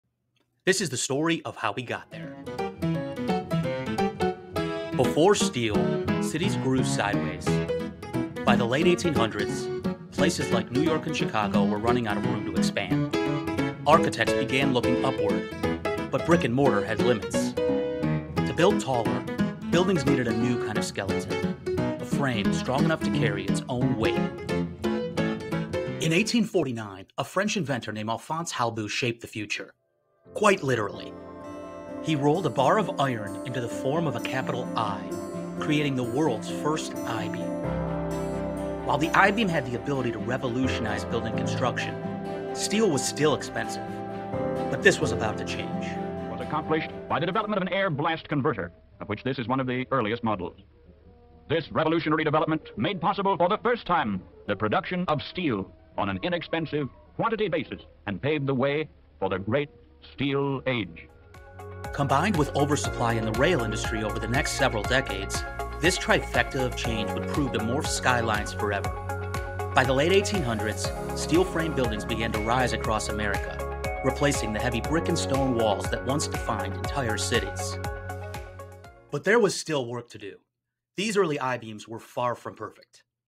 From I Beams to the W14x1000 - The Structural Steel Evolution (Part 2)
On July 1, 2025, history was made in Detroit.

At the Henry Ford Health project, crews set the W14×1000 — the heaviest rolled wide-flange steel section ever produced. At 1,000 pounds per foot, it represents the culmination of more than 175 years of structural steel innovation.

This video traces that evolution — from the first I-beams of the 19th century, through Henry Grey’s universal mill, Bethlehem Steel’s wide-flange revolution, the steady growth in size and strength through AISC standards, and finally to ASTM A913 high-strength steel produced at ArcelorMittal Differdange.

The W14×1000 isn’t just a record-setting shape.
It’s the modern answer to a problem structural engineers have been solving since the earliest skyscrapers: how to carry more load with fewer, simpler, more efficient structural members.